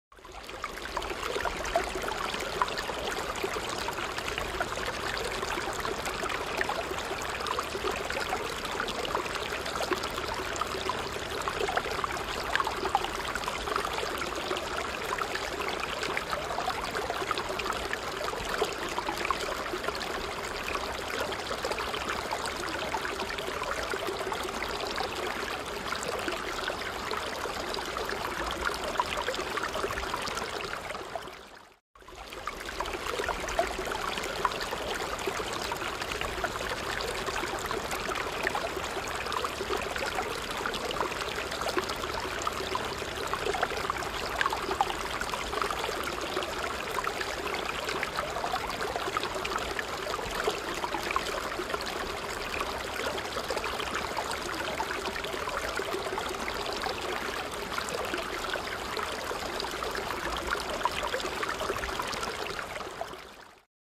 Suara Air Gemericik - Pengantar Tidur, Nyaman dan Damai
Suara Air Gemericik cocok untuk pengantar tidur. Suara air sungai mengalir di hutan, damai dan menenangkan.